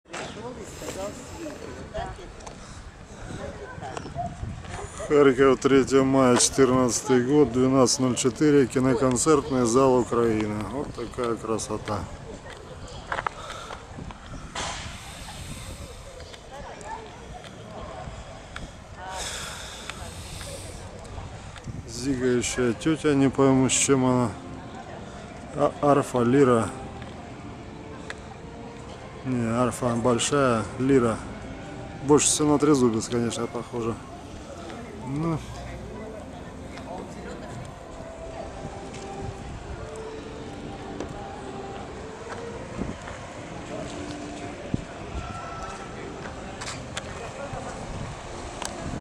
ККЗ "Украина" и мозаика зигующей тёти с трезубцем, Харьков 03.05.2014
Один из двенадцати официальных символов Харькова - Киноконцертный зал "Украина".  Мозаика то ли девушки указывающей путь с лирой (не валютой) , то ли зигующей девушки с трезубцем. Сад Шевченко 3 мая 2014 год.